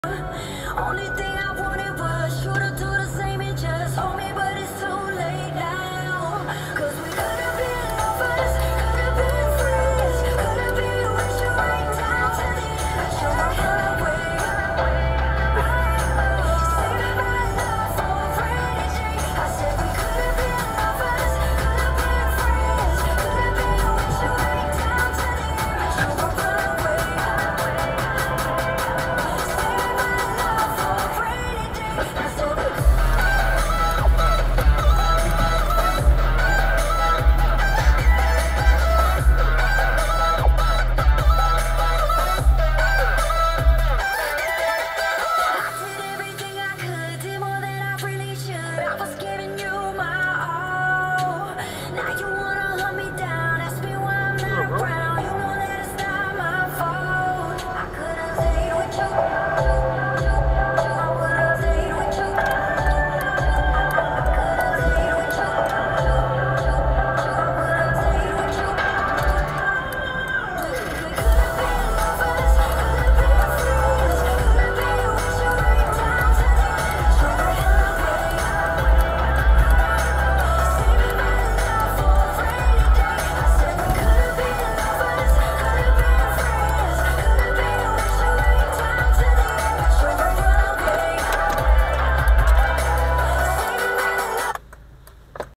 Spider crawling over my Dash Cam
good thing in my trunk I keep a handy roll of paper towels, a filled water bottle, and incendiary grenade